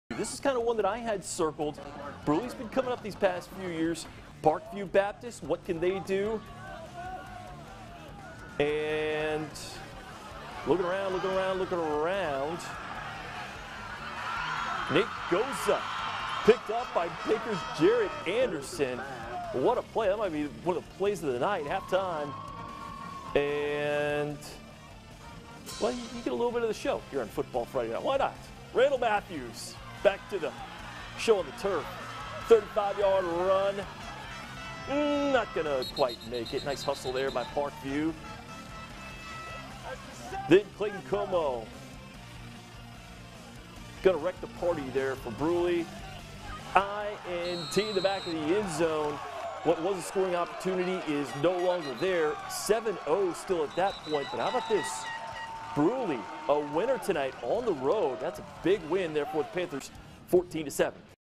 Brusly edges Parkview for seven-point win
The Panthers won a close game against the Eagles.